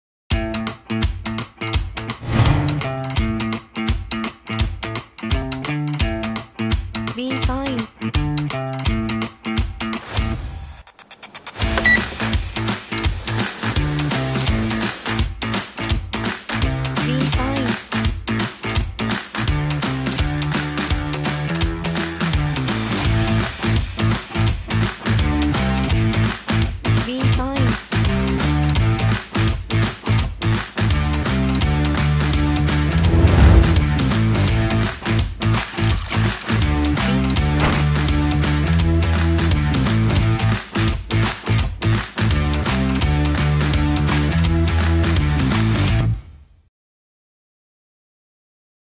UWANT Multiple spot cleaner
UWANT multiple spot cleaner B100-E---Special and professional designed for carpet, curtain and fabric sofa cleaning. It is leading an innovative way of fabric cleaning ---Spray+Scrub+Vacuum+Drain four in one to ensure deep cleaning with quick drying just like your removable washing machine.